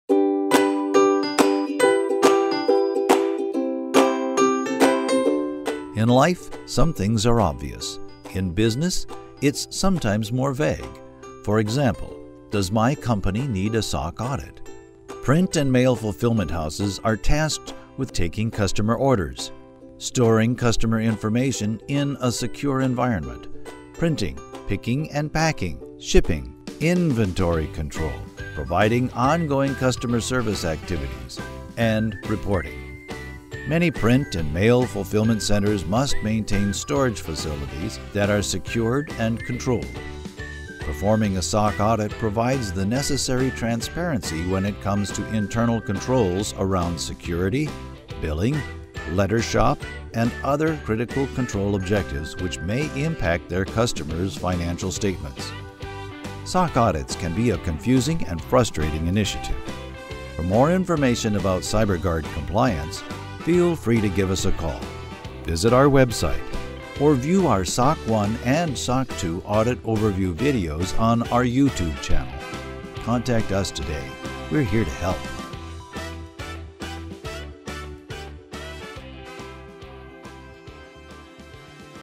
CyberGuard Compliance Print Mail Fulfillment Houses and the Need for a SOC Audit
Print and mail fulfillment houses are tasked with taking customer orders, storing customer information in a secure environment, printing, picking & packing, shipping, inventory control, providing ongoing customer service activities, and reporting. Many print and mail fulfillment centers must maintain storage facilities that are secured and controlled. Performing a SOC audit provides the necessary transparency when it comes to internal controls around security, billing, lettershop, and other critical control objectives which may impact their customers’ financial statements.

CyberGuard is based in the United States and serves clients around the globe.

CGCompliance.com